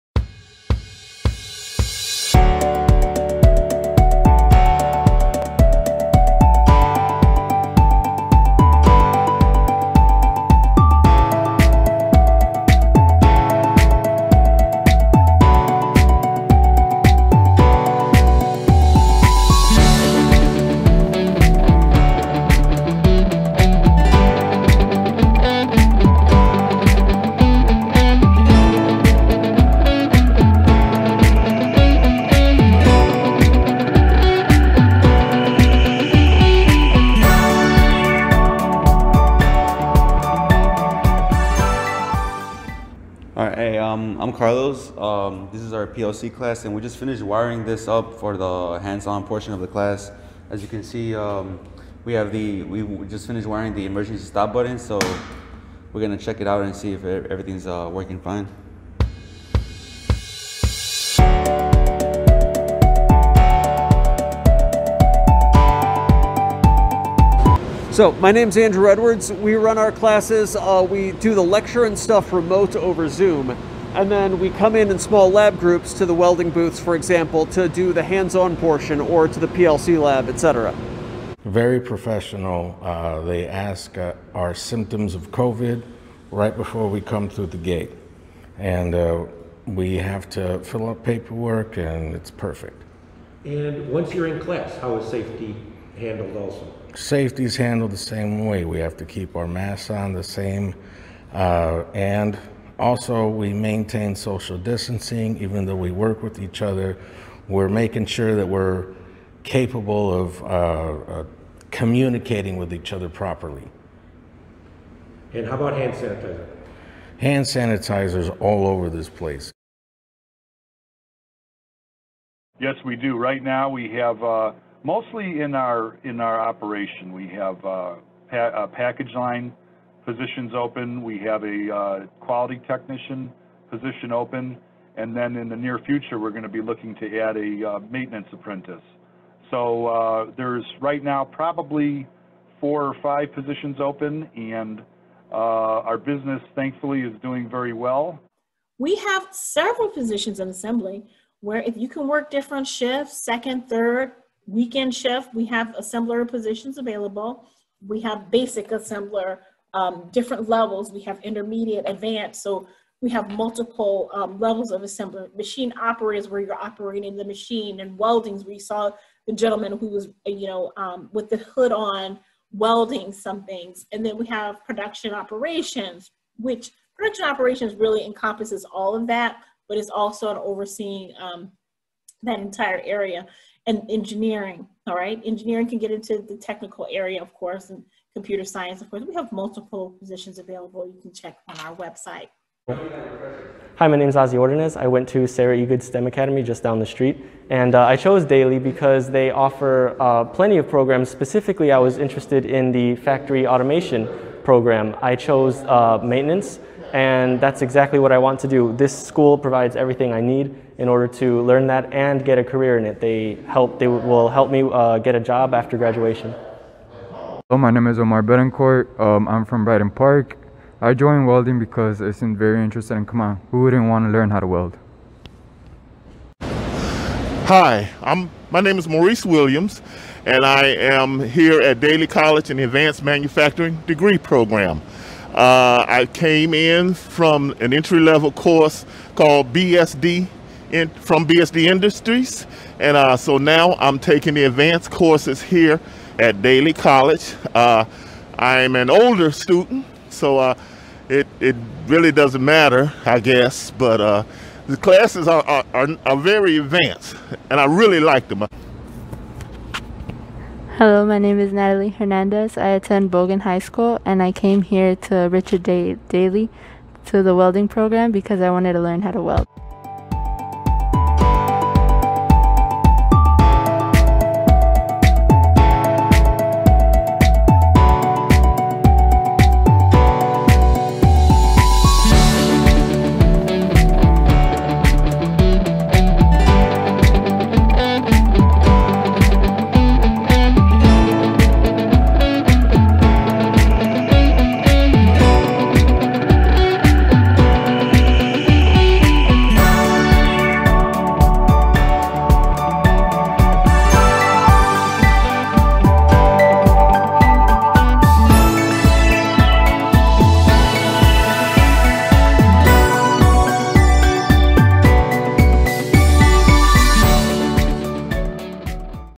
Overview of Manufacturing Program at Richard J. Daley College
Learn more about our Manufacturing program from students, faculty and industry partners.